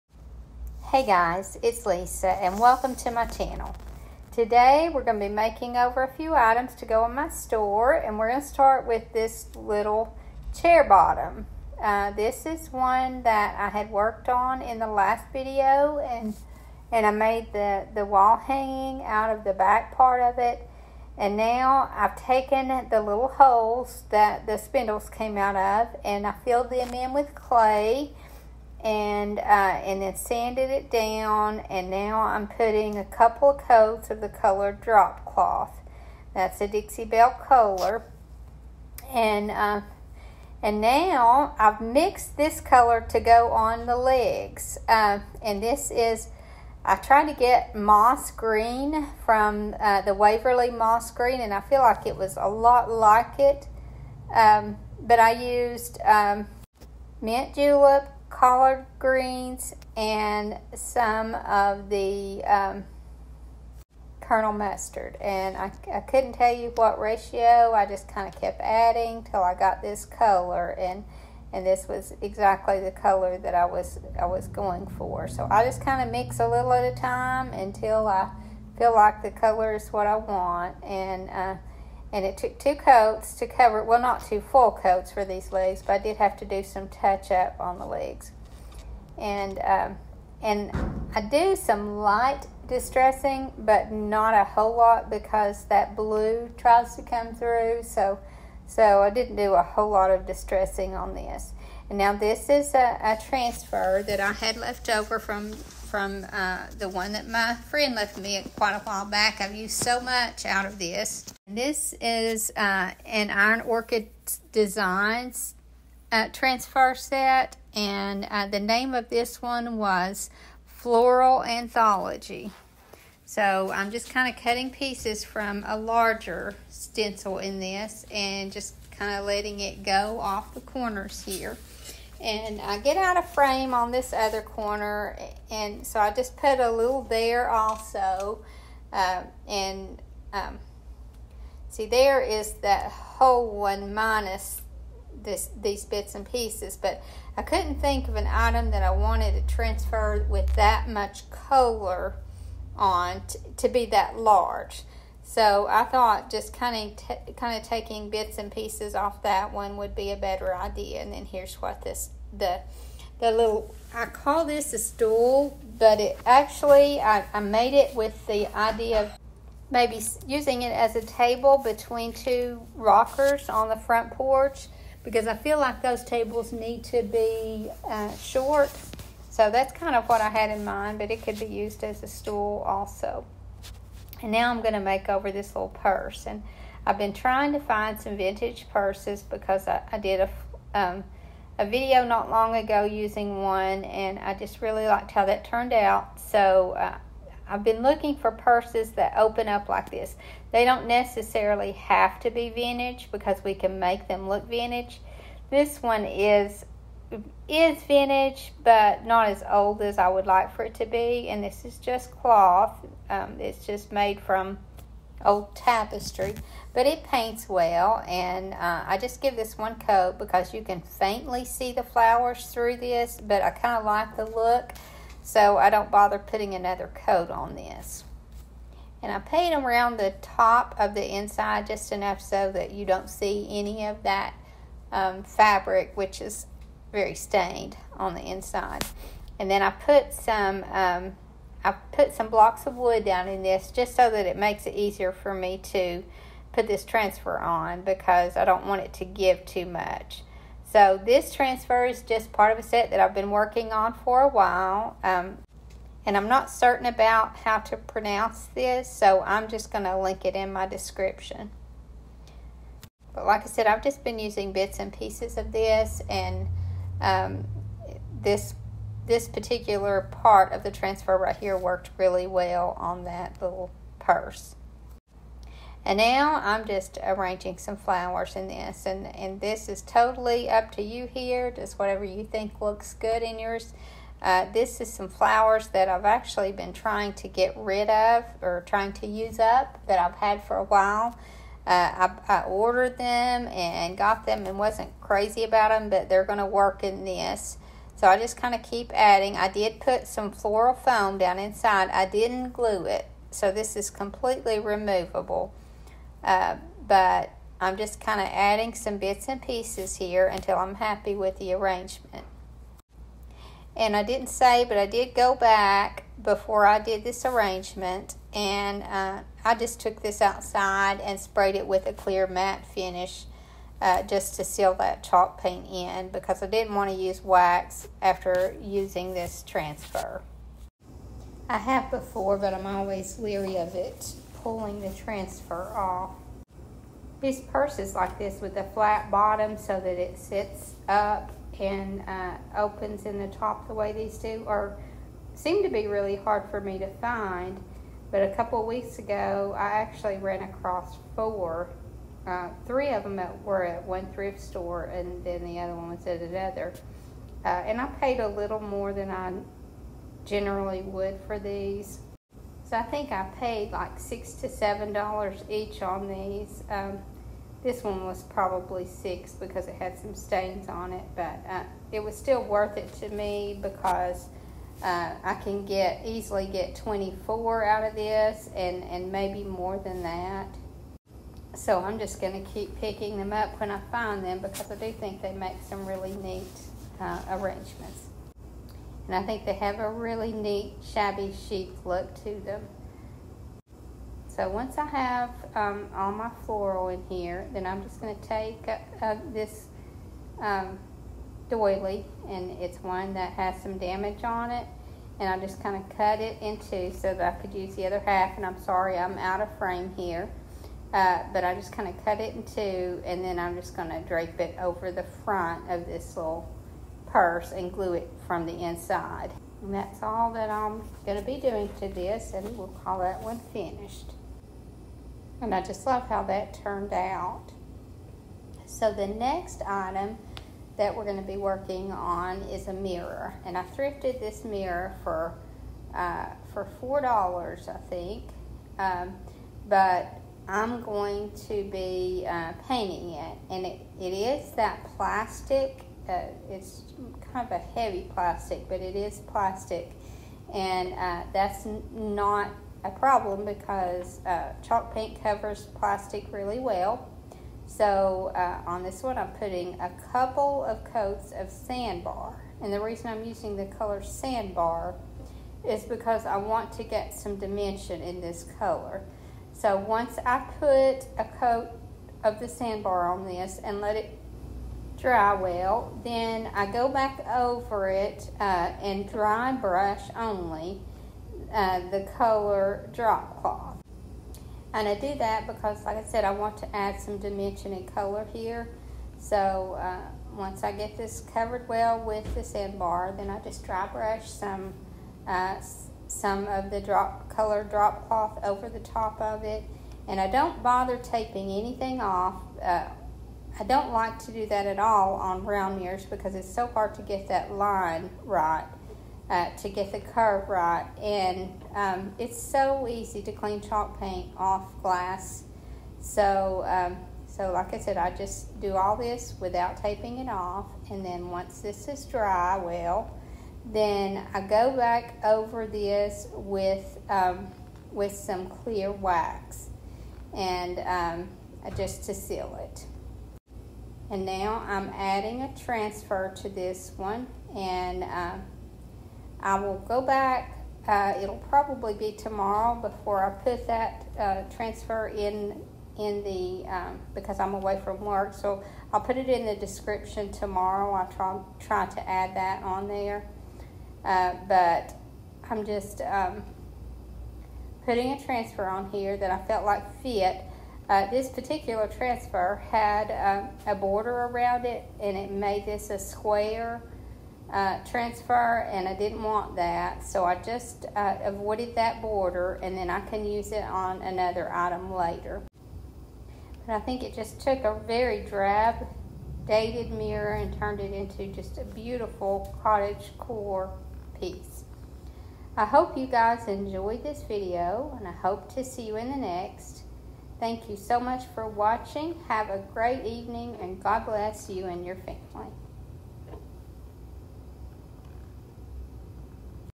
How to Create Cottagecore Items from outdated Thrift finds
How to upcycle thrifted items to fit your cottage decor
Label Ephemera transfer by IOD